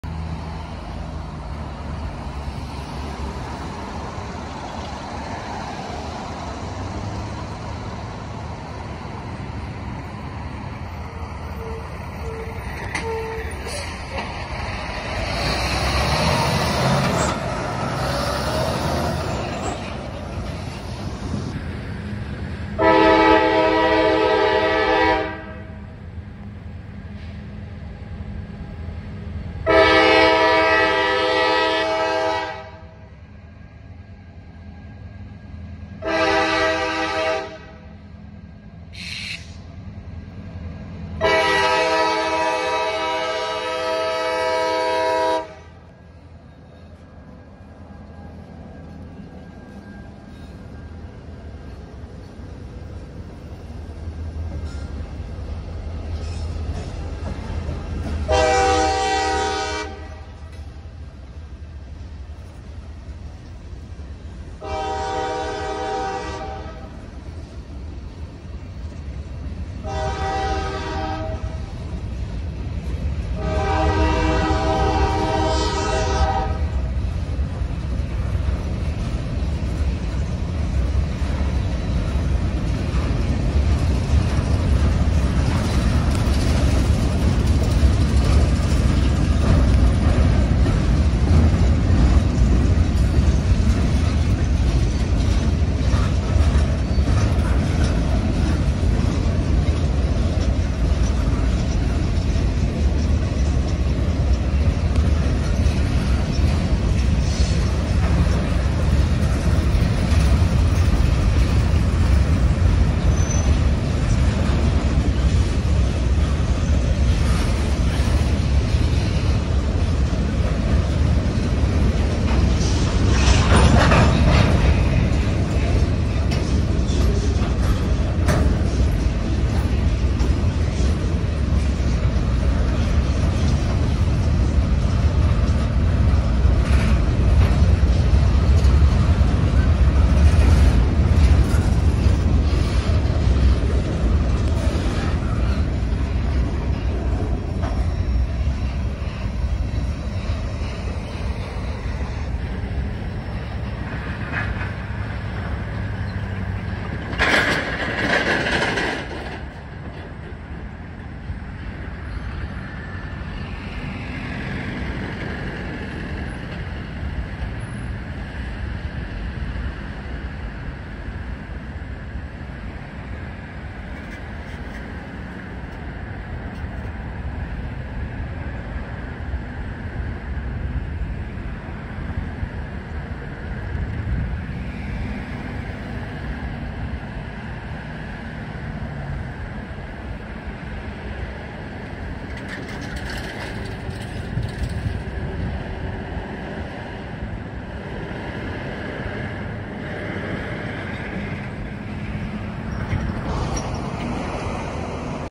CSX L135 in New Oxford - 9/2/22
CSX local freight train L135 is seen crossing Lincoln Way (U.S. Route 30) on the Hanover Subdivision with a Nathan K5LA horn in New Oxford, Pennsylvania at 11:16 AM on Friday, September 2, 2022. The engines in this video are: CSX 2610 (ex-Seaboard GP38-2), CSX 7903 (D8-40CW) and CSX 4015 (SD40-3). Enjoy!